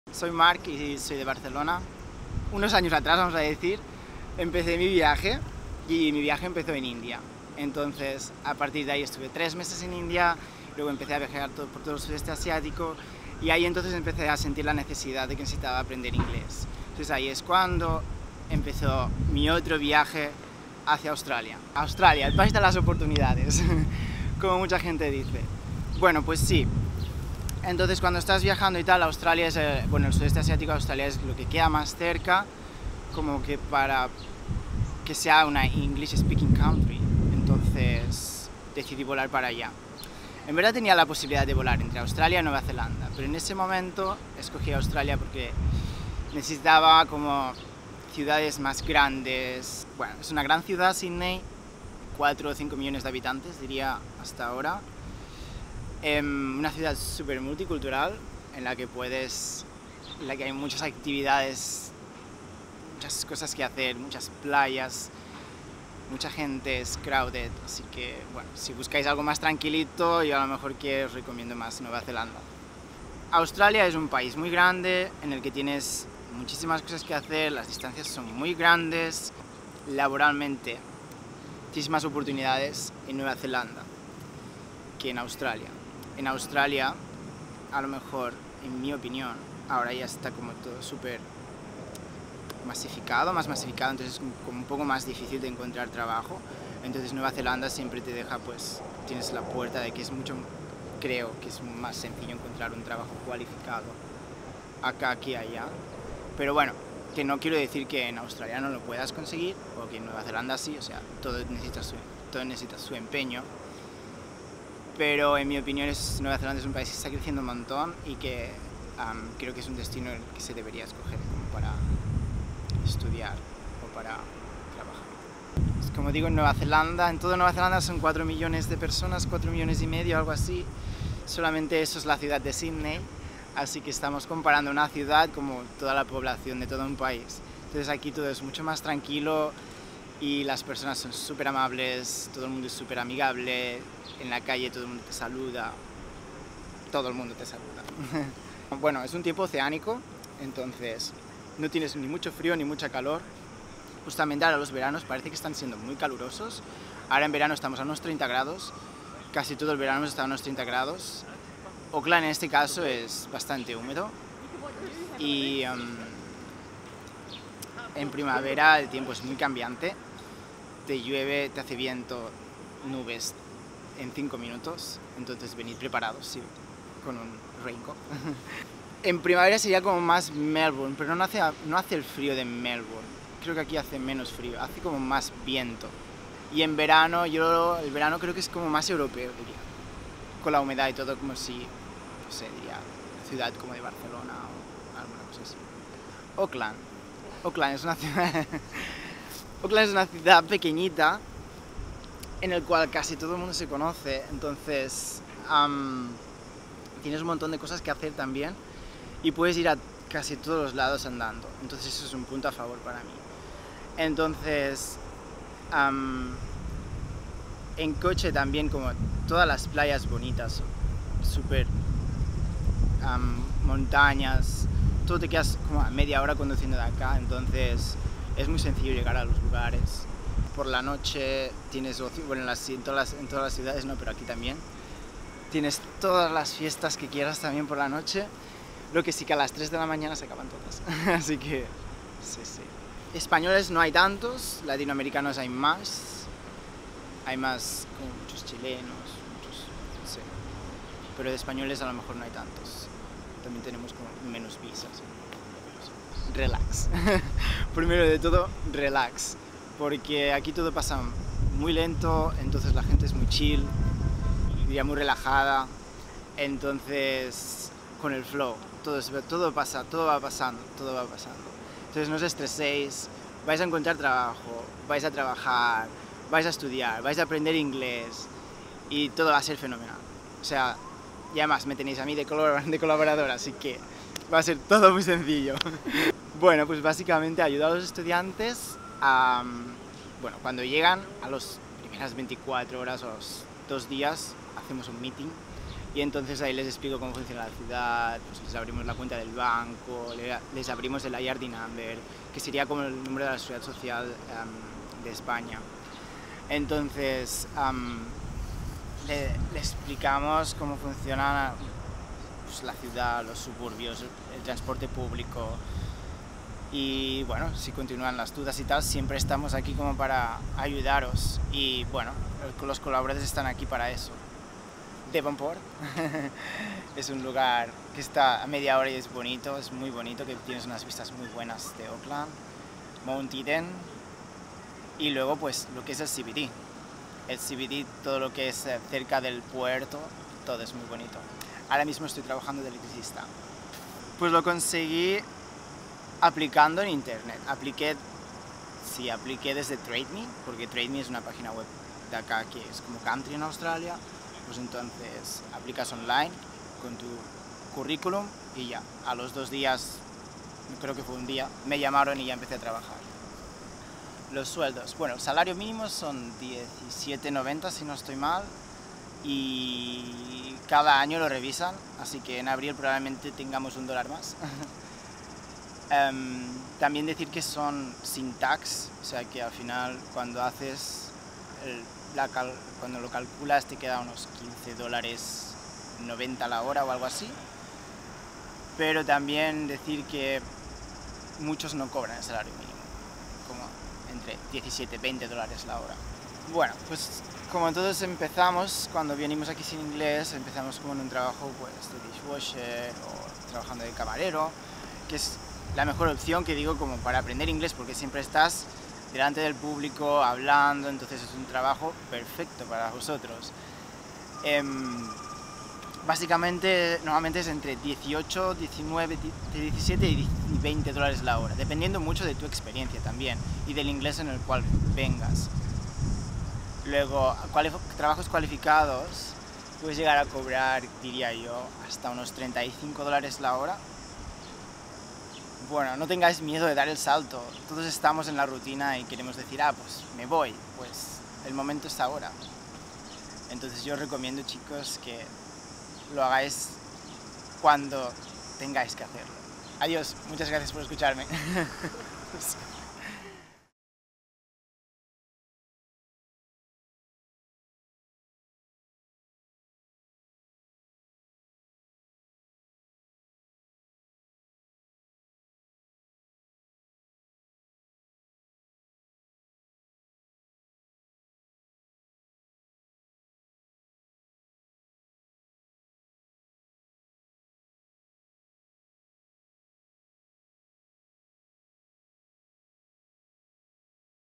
Entre Nueva Zelanda y Australia - Marc La Rosa
Marc es un chico de Barcelona que empezó su viaje en la Índia y siguió por el sureste asiático. Viajando se dio cuenta que necesitaba mejorar su inglés y decidió volar hasta Australia, que le quedaba más cerca, para estudiar.

Durante unos meses estudió en la ciudad de Sydney y luego siguió su aventura en Auckland, Nueva Zelanda. En esta entrevista nos cuenta las diferencias que, según su opinión, hay entre los dos países: oportunidades de trabajo, ciudades, estilo de vida, clima...

¿Quieres vivir una experiencia como la de Marc? ¡Escríbenos!

Estas son las preguntas:

1. ¿Por qué fue Australia tu primer destino para aprender inglés? Minuto 00:23
2. ¿Qué tal Sídney? Minuto 00:50
3. ¿Diferencias entre Australia y Nueva Zelanda? Minuto 01:12
4. El clima de Auckland. Minuto 02:30
5. ¿Cómo es Auckland? Minuto 03:17
6. ¿Hay hispanohablantes en Auckland? Minuto 04:02
7. Consejos para los recién llegados? Minuto 4:17
7. ¿Cuál es tu trabajo como COOLaborador? Minuto 4:47
8. ¿Qué nos recomiendas visitar? Minuto 5:27
9. ¿Cuál es tu trabajo y cómo lo conseguiste? Minuto 5:44
10. El salario mínimo de Nueva Zelanda. Minuto 06:05
11. ¿De qué trabajan los estOOdiantes en Nueva Zelanda? 06:40

¡Muchas gracias Marc!